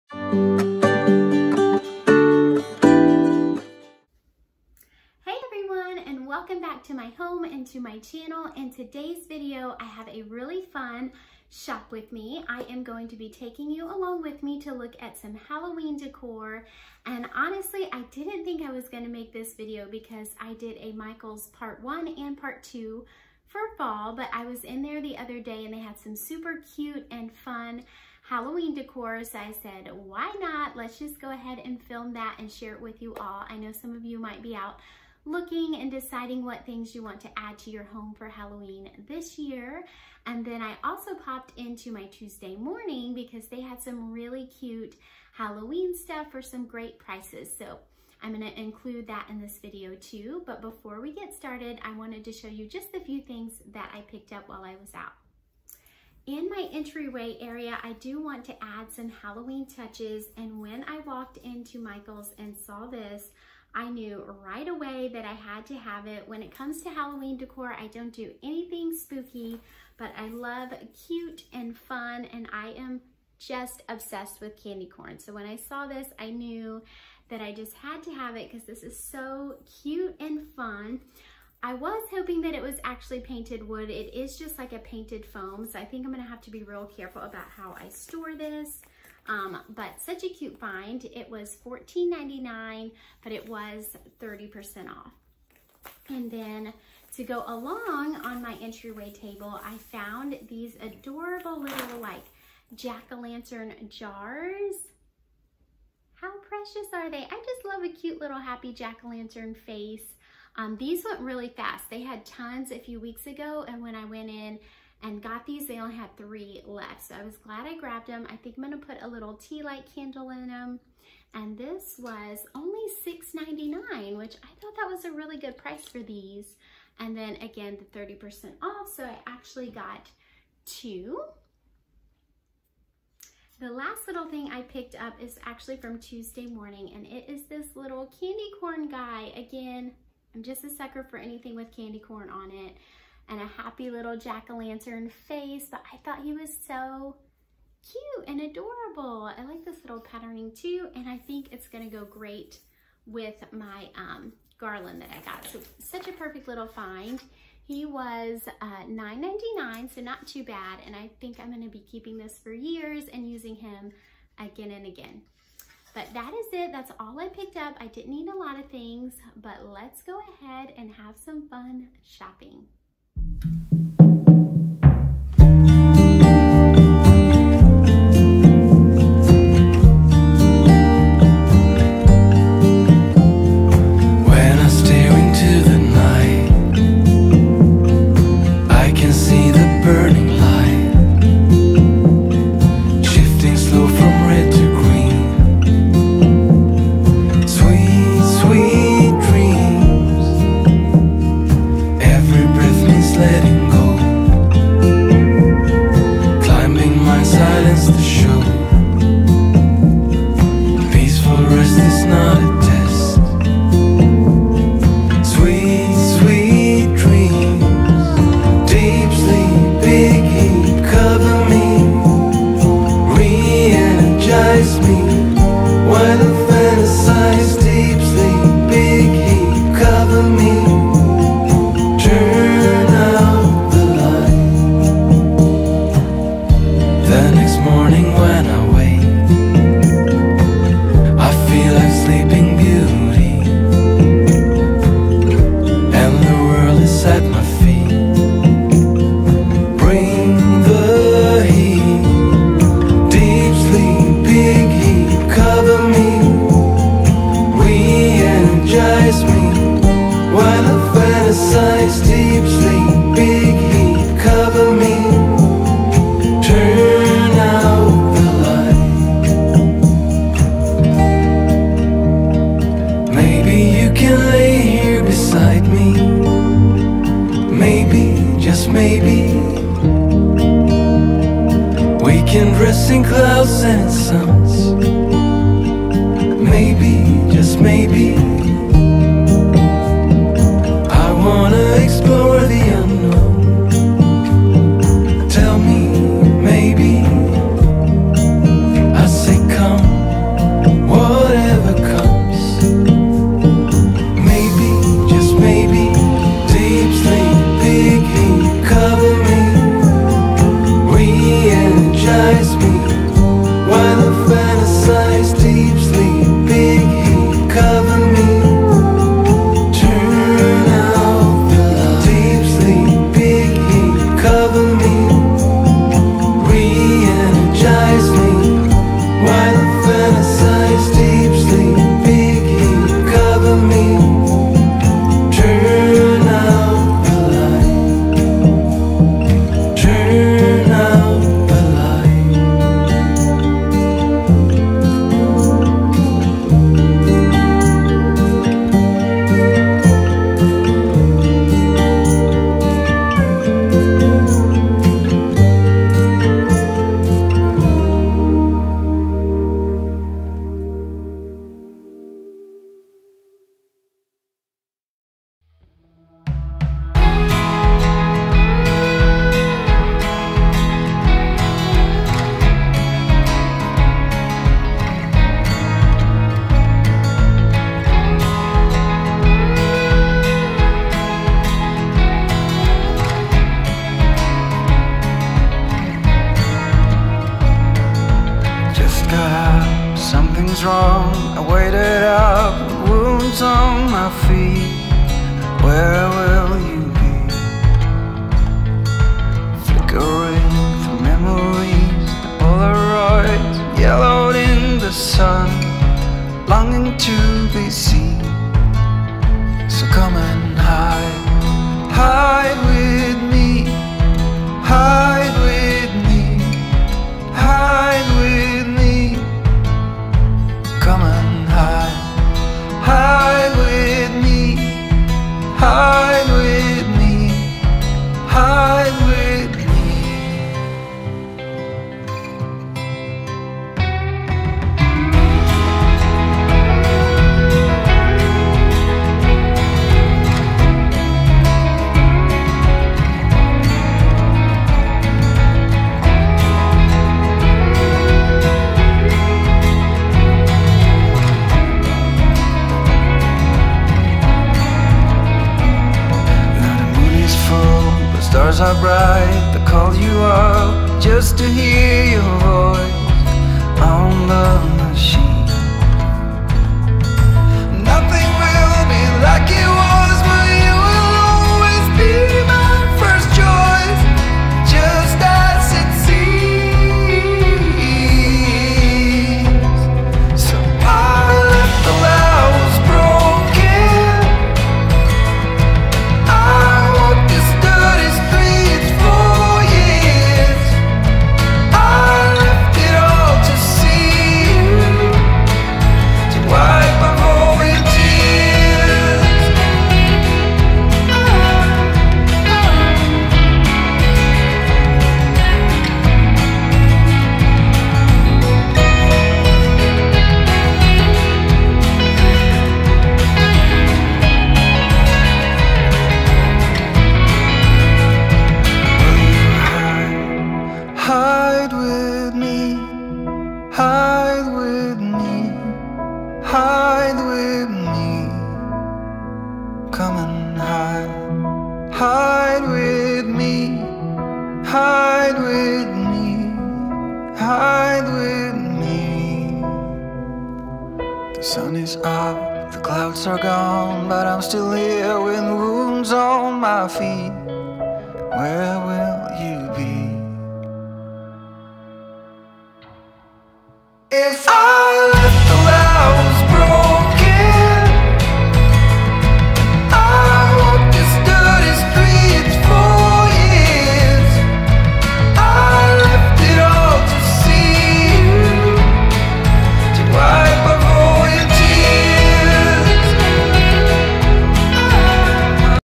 2020 HALLOWEEN SHOP WITH ME // MICHAELS AND TUESDAY MORNING
Hey y'all!! Anyone else super excited that Halloween is right around the corner?! I have started shopping and putting out the cute and fun Halloween decor! Michaels and Tuesday morning had the best Halloween decor finds! Sit down, relax, and let's go shopping together!

Channel Art